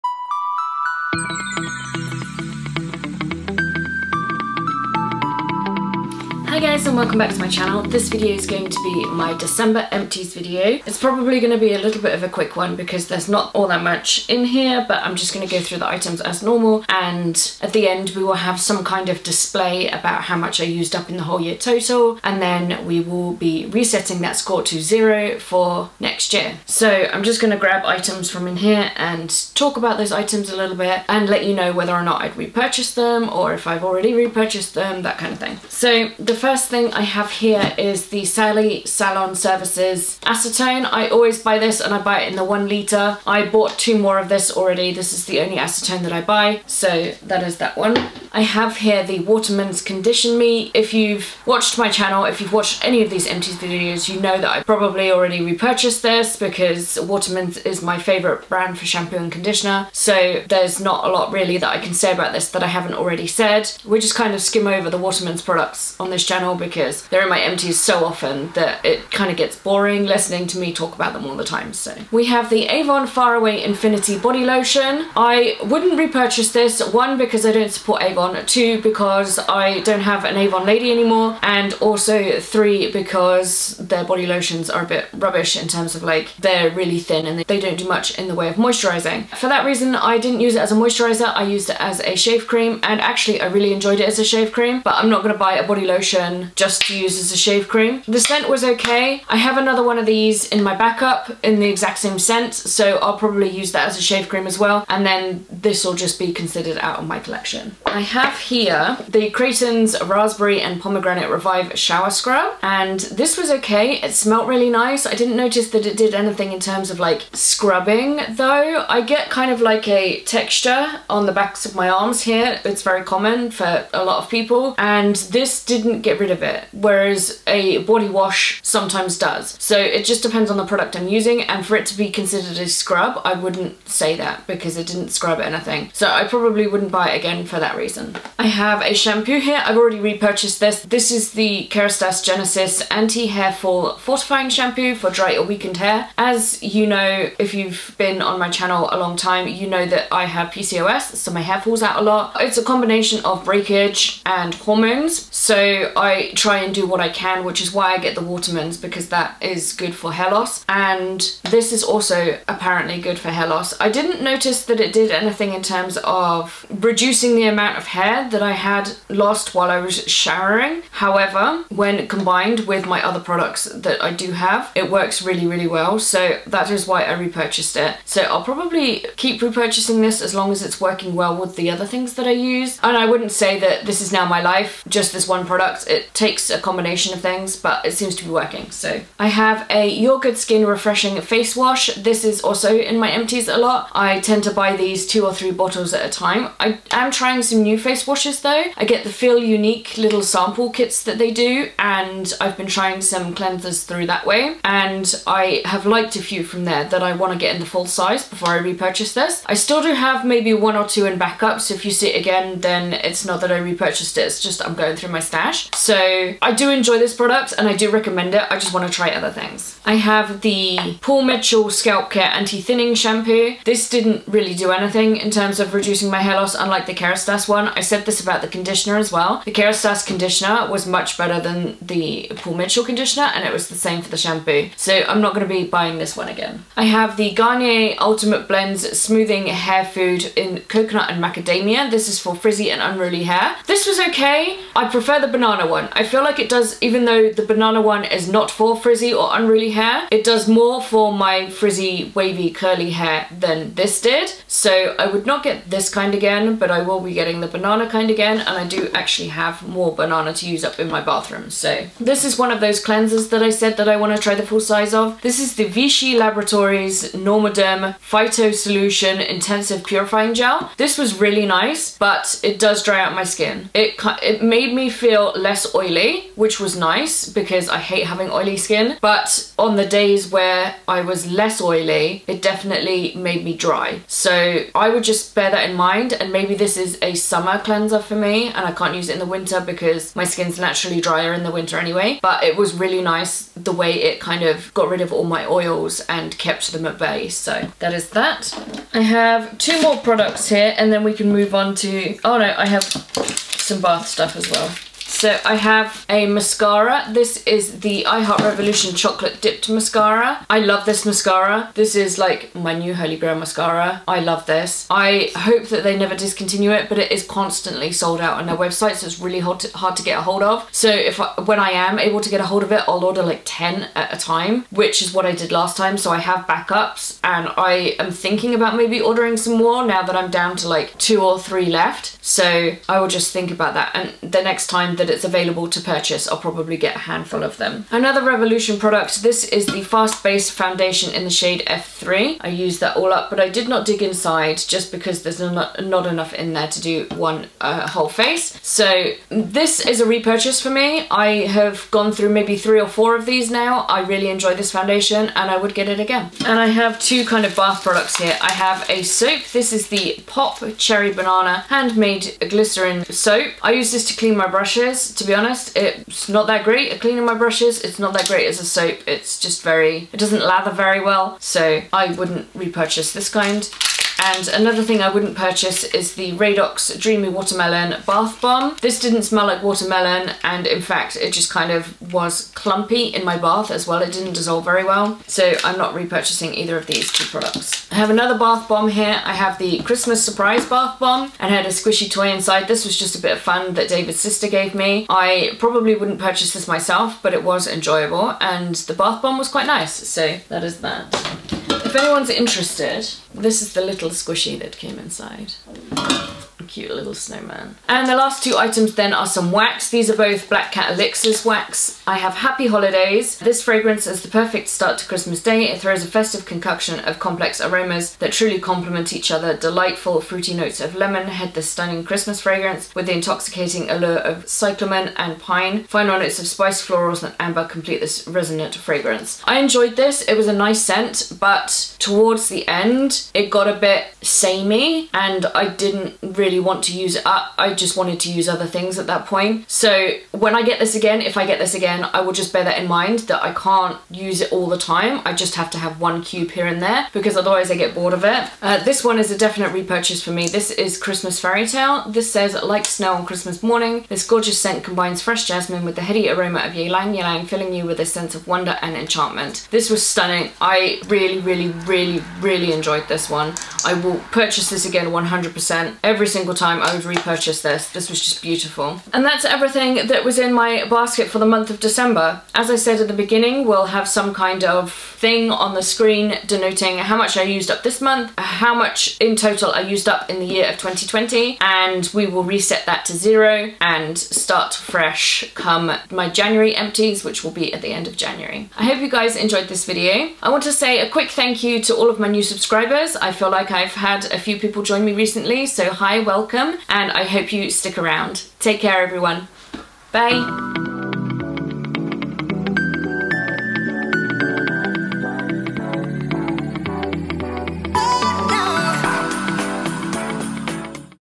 December Beauty Empties
Here's my beauty trash for the month of December.